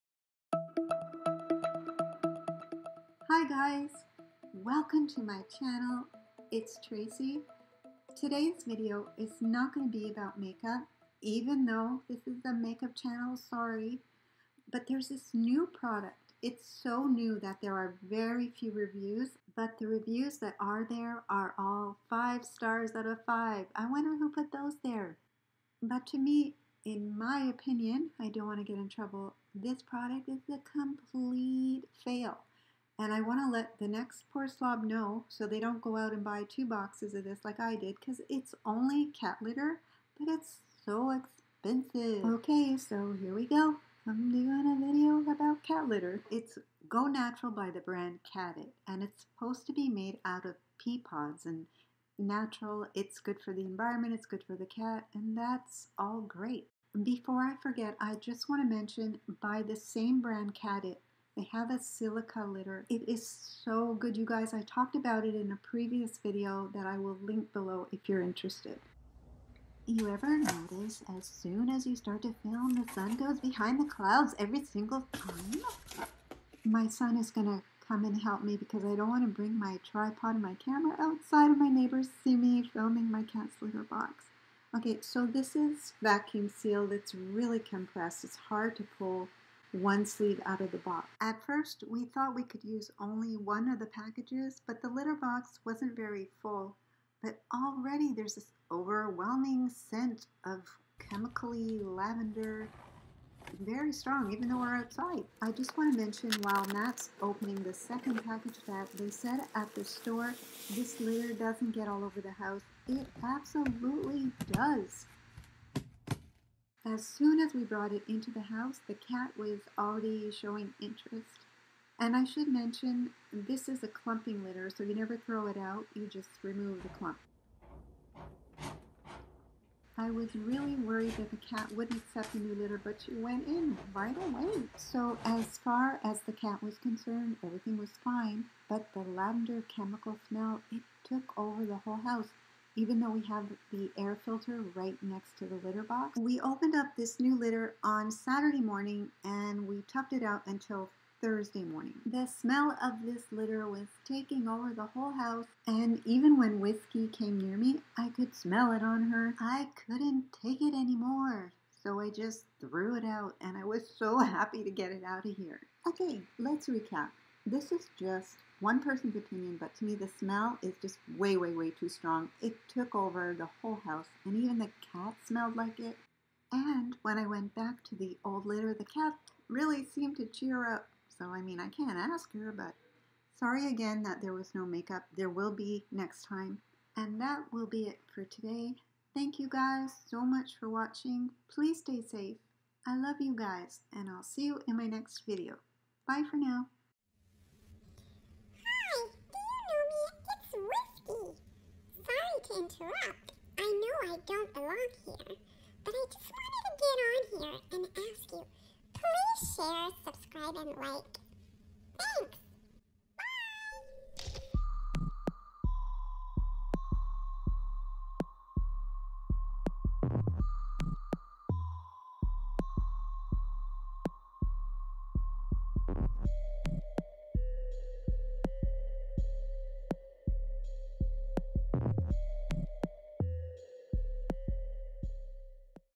CATIT GO NATURAL PEA HUSK CAT LITTER PRODUCT REVIEW
🌸🌺🌼 HI friends, sorry this is not my usual but if I can save one person from buying this _____________ (fill in blank with swear words) litter, this video will be worth it.🌸🌺🌼